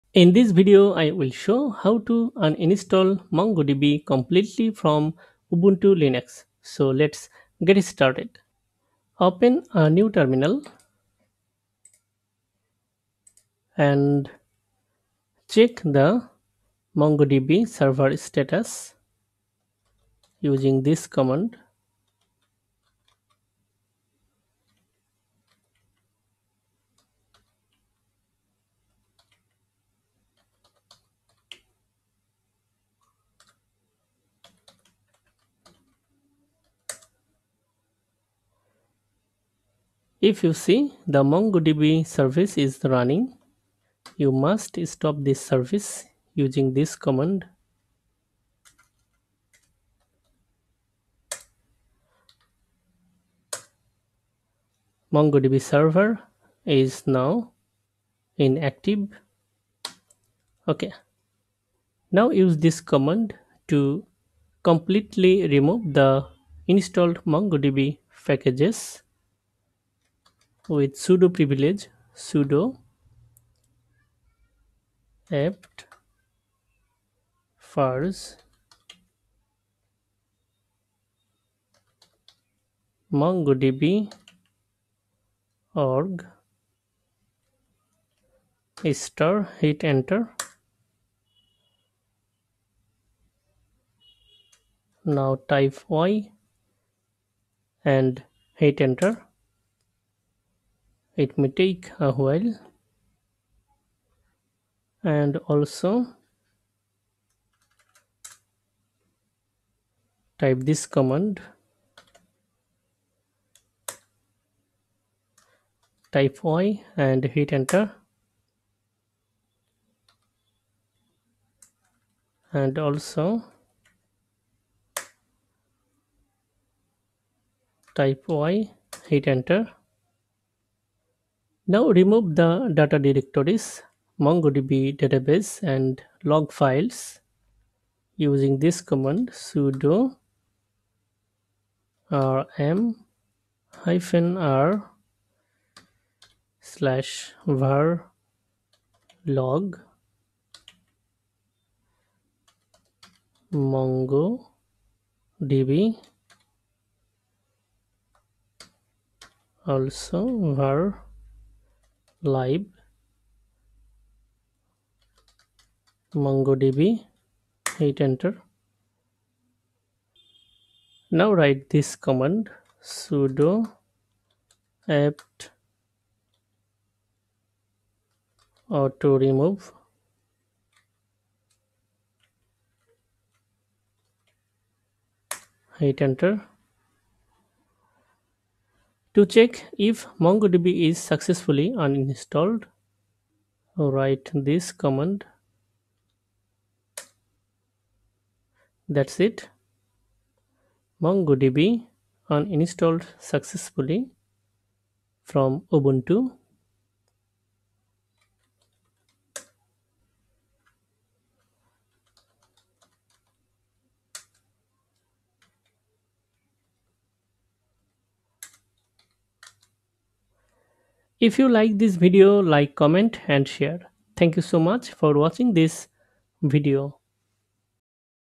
How to uninstall MongoDB completely from Ubuntu Linux
In this video I will show How to uninstall MongoDB completely from Ubuntu Linux.